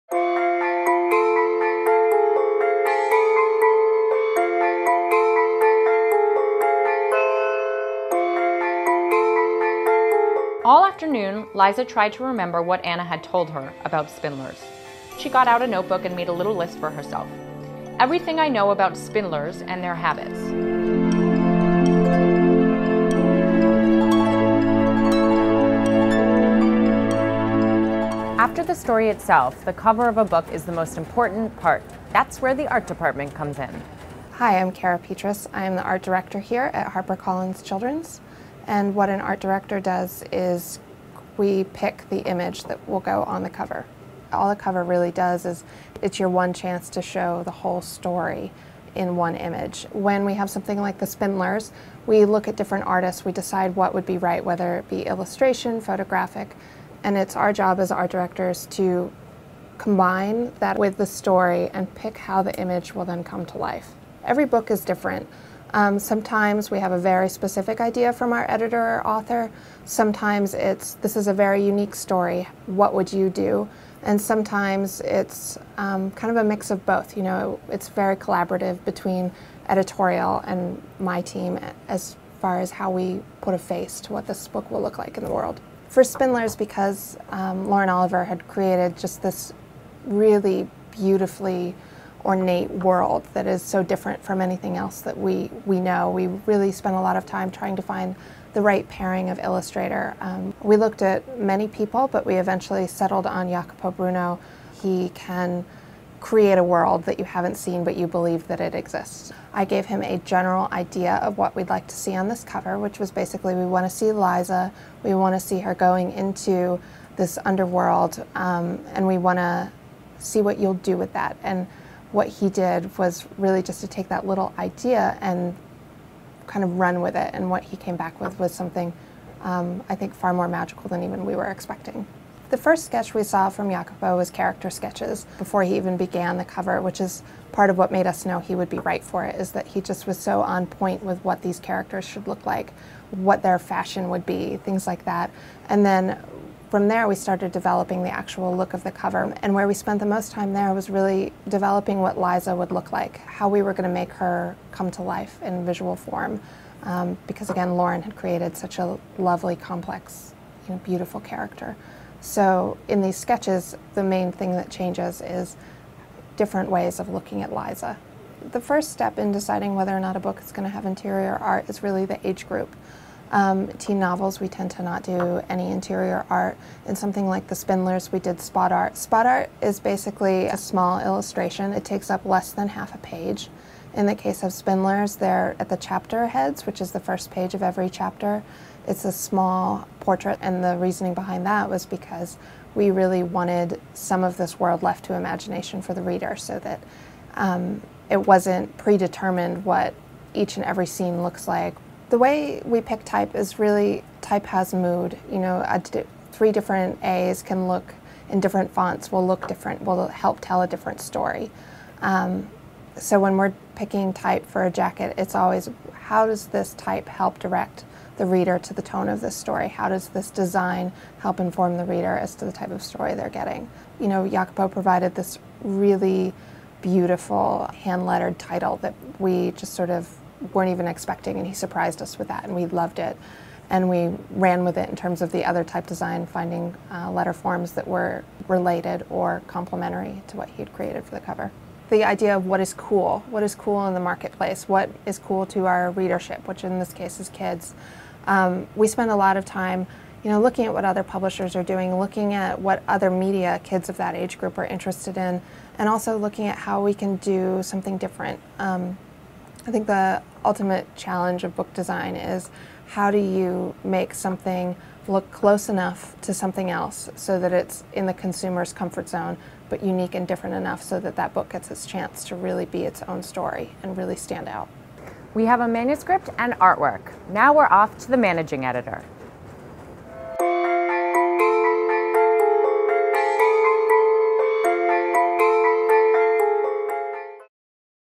HOW A BOOK IS MADE - EPISODE 4: CREATING THE ART
While at the offices of HarperCollins Publishers, Lauren Oliver visits the book design department to learn how book covers are created. In this episode, Art Director Cara Petrus explores the collaboration between the design and editorial teams, the process of selecting an artist, and the use of typeface to complement the tone of a book. Along the way, viewers will see the artwork for THE SPINDLERS evolve from initial concept sketches into a finished book jacket!

ABOUT THE SPINDLERS:
When Liza's brother, Patrick, changes overnight, Liza knows exactly what has happened: The spindlers have gotten to him and stolen his soul. She knows, too, that she is the only one that can save him.

To rescue Patrick, Liza must go Below, armed with little more than her wits and a broom. There, she uncovers a vast world populated with talking rats, music-loving moles, greedy troglods, and overexcitable nids . . . as well as terrible dangers. But she will face her greatest challenge at the spindlers' nests, where she encounters the evil queen and must pass a series of deadly tests — or else her soul, too, will  remain Below forever.